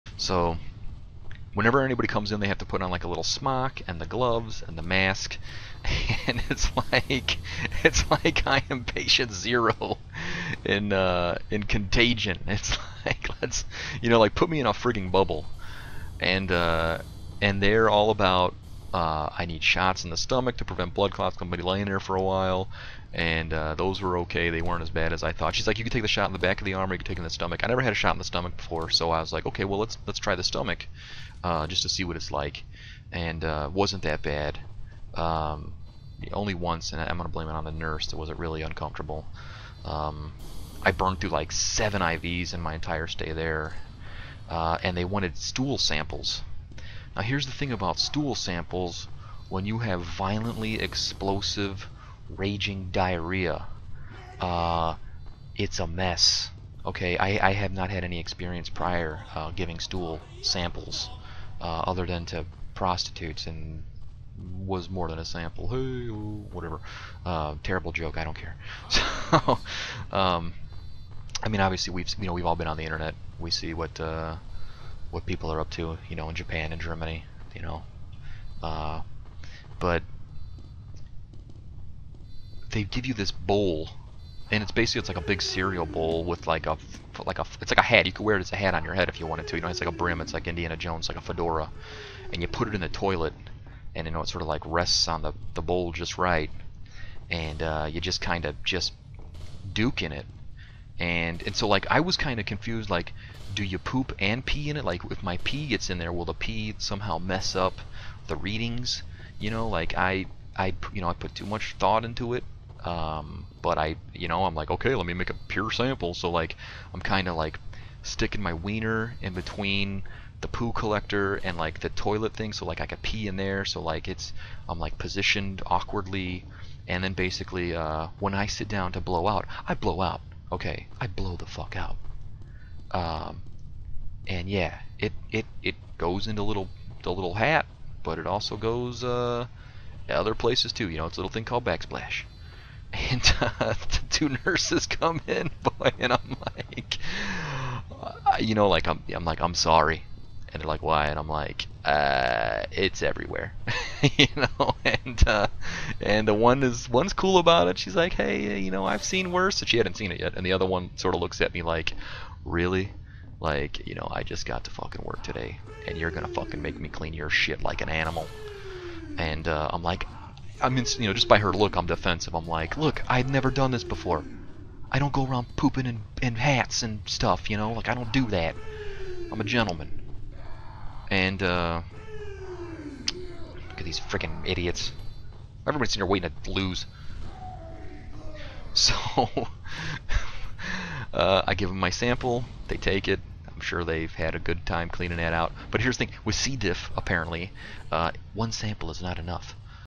New DB Chronicle Teaser! Welcome Back!
Holy crap! (literally). So here's a little teaser to the TWO part story of my near demise that took place while I SHOULD have been taking my final exams. Absolute nightmare of epic proportions... so I figured who better to share with than all of you fine people.